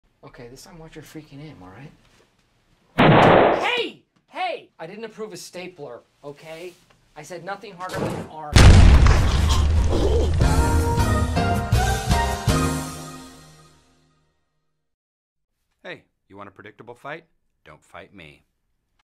Aqua Teen Office Force
Dwilock uses an exploding stapler on Master Michael for siding with the stanfordnites